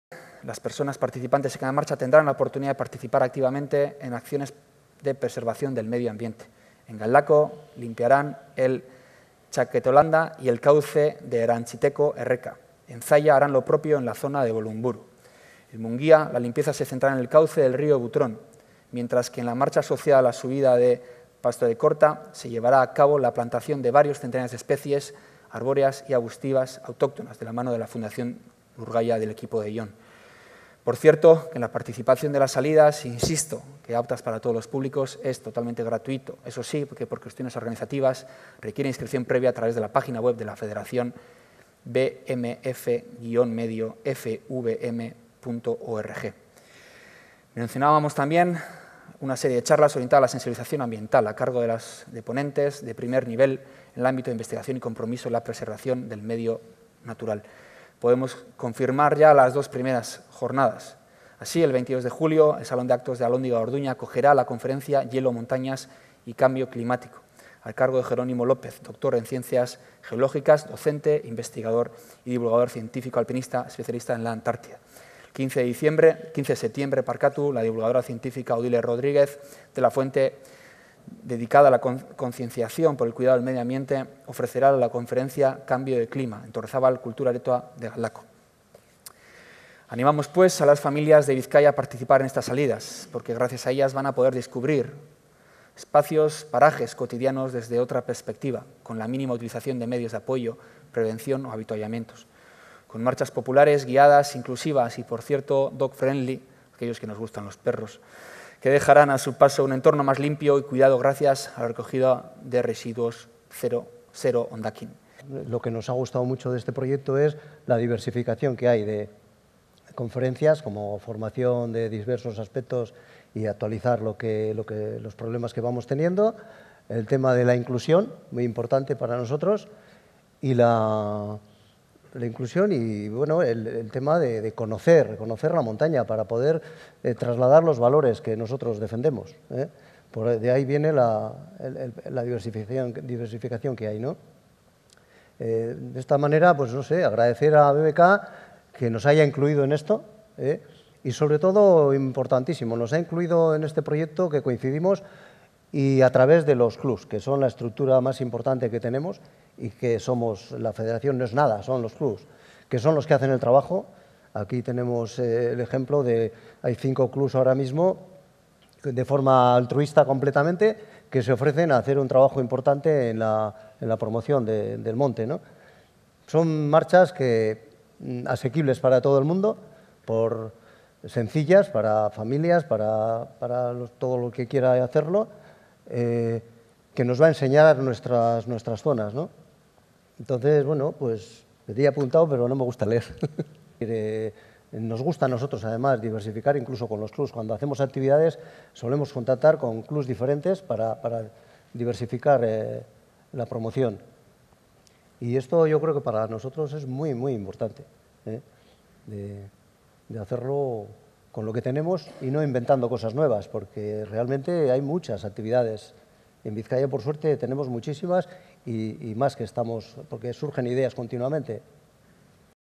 BBK organiza 5 marchas guiadas de montaña por Bizkaia
BBK Mendi Martxak se desarrollará de julio a diciembre con subidas a  Txarlazo, Ganguren, Ubieta, Tallu y Pastorekorta

Todas las marchas incluirán actividades paralelas como conferencias relacionadas con la investigación y la preservación del medio natural, limpieza de arroyos o zonas concretas, plantación de especies autóctonas y la instalación de puestos de productos autóctonos.

BBK cuenta con la colaboración de clubes de diferentes comarcas y con Bizkaiko Mendi Federazioa.